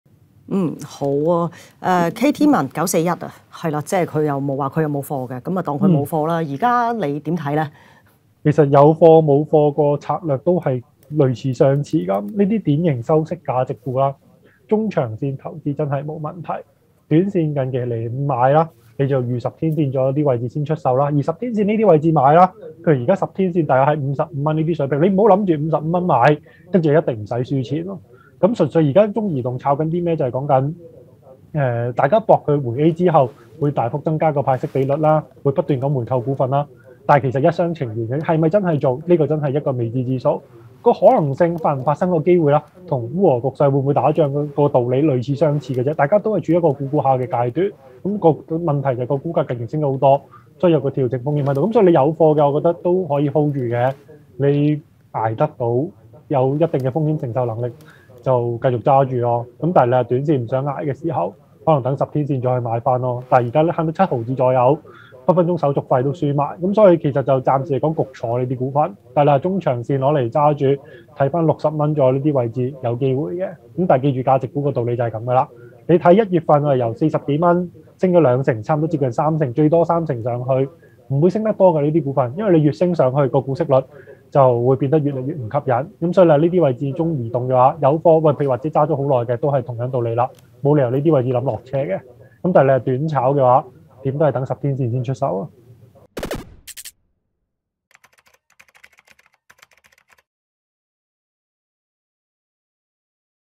【開市追揸沽】（精華）：中移動（00941）點睇？ 短炒何時出手？
中移動（00941）今天收市跌近2%報55.25元，此典型收息價值股早前上試58.65元最高位後開始回調，超買下暫仍企於10天綫55元之上，若明日一旦跌穿會引發更深回調？耀才證券研究部經理譚智樂（Arnold）為大家分析最佳短炒入市價位。立即點擊收看／收聽→【精華速睇】

**重溫【ET開市追揸沽】港股現水平應否做淡？ 恒指周五季檢前對港股影響**